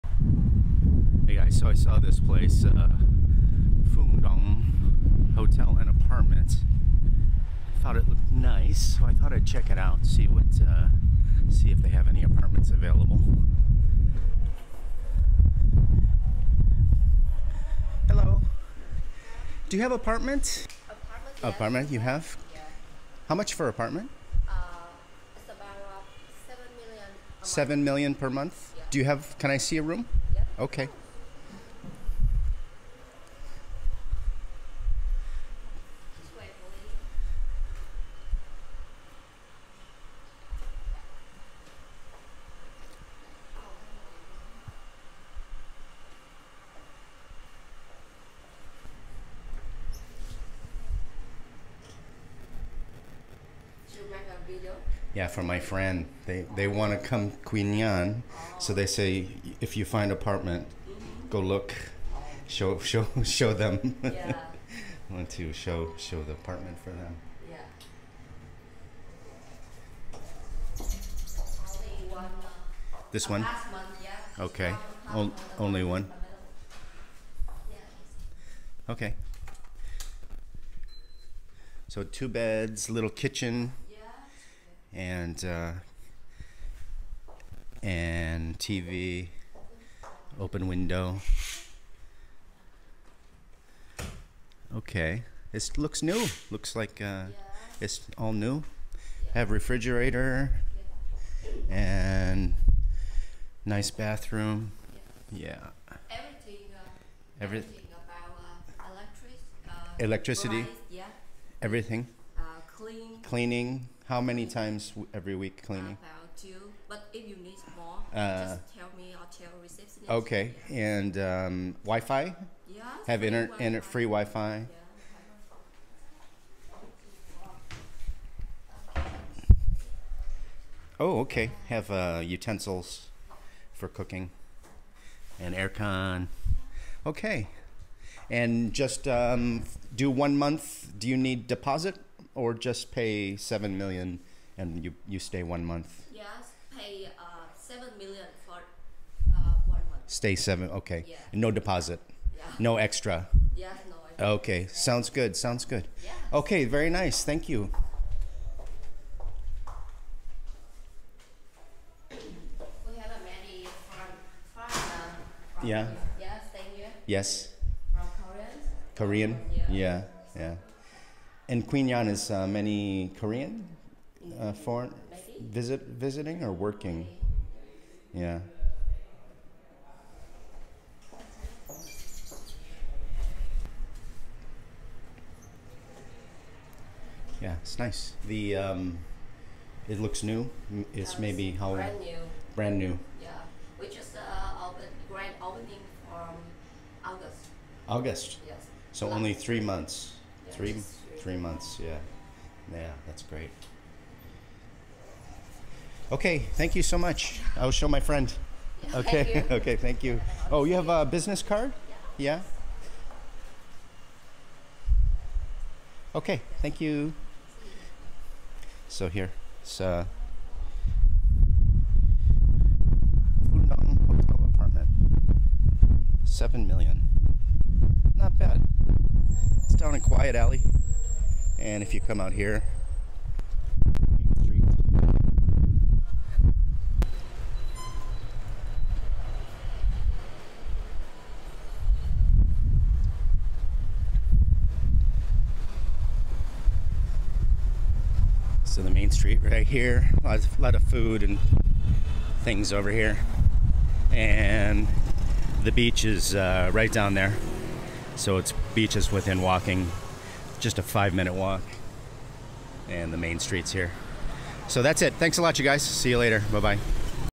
A look at a serviced apartment in Quy Nhon, Vietnam
Today I check out a serviced apartment in Quy Nhon.